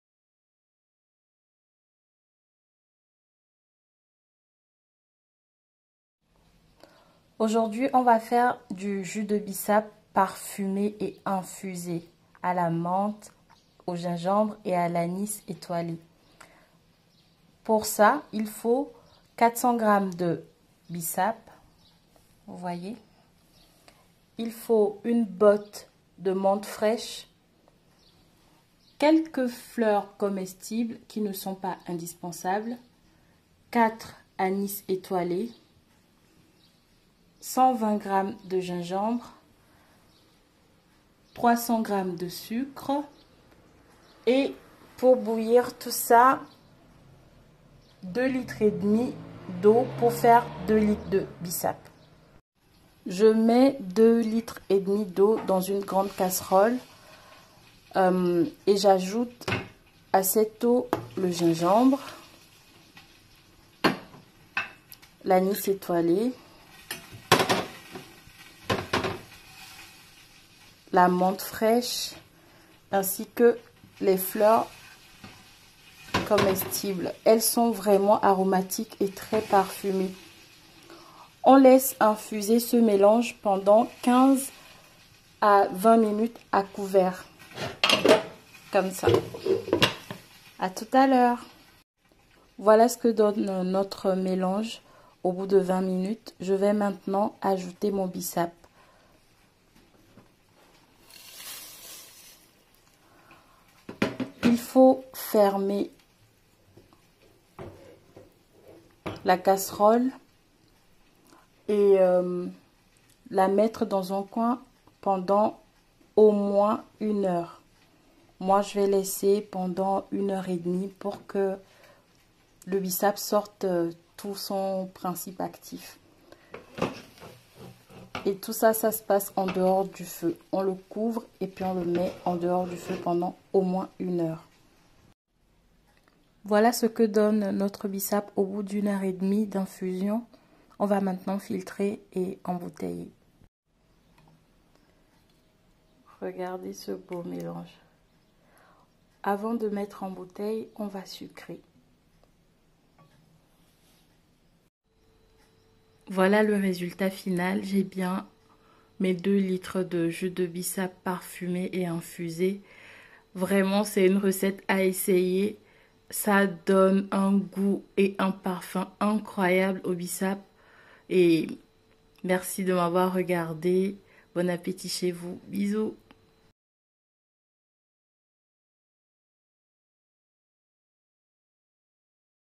Jus de Bissap infusé, jus de Foléré, jus de fleur d'hibiscus Recette_revisitée 1 MKS 5
Découvrez une autre manière de faire votre jus de Bissap avec un goût et un parfum incroyables grâce à une infusion à la menthe fraîche,  au gingembre et à l'anis étoilé.

Ingrédients
- 400g de Bissap ou fleur d'hibiscus
- 1 botte de menthe fraîche
- 4 anis étoilés
- 400g de gingembre
- 2,5 l d'eau
- 300g de sucre